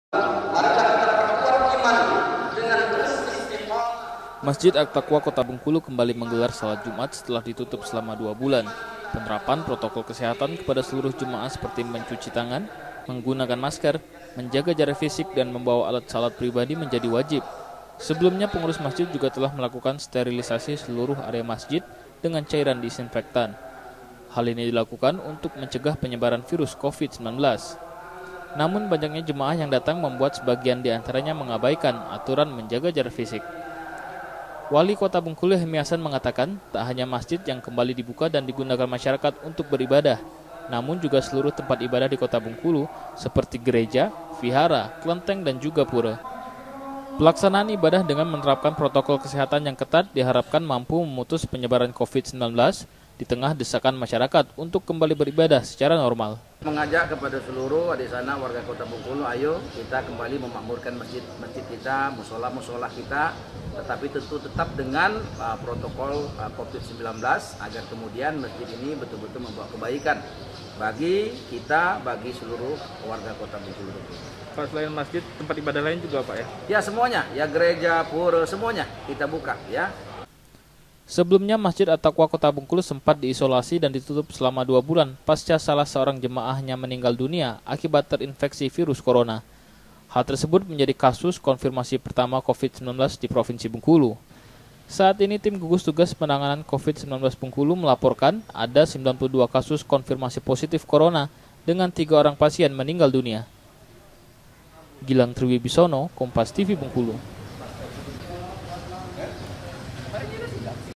MASJID AT TAQWA GELAR SHALAT JUMAT PERDANA
SEMPAT DITUTUP SELAMA 2 BULAN DEMI MENCEGAH PENYEBARAN COVID-19, MASJID AT TAQWA KOTA BENGKULU KEMBALI DIBUKA. DALAM SHALAT JUMAT PERDANA INI, SELURUH JEMAAH DIWAJIBKAN MENGIKUTI PROTOKOL KESEHATAN SESUAI ANJURAN PEMERINTAH.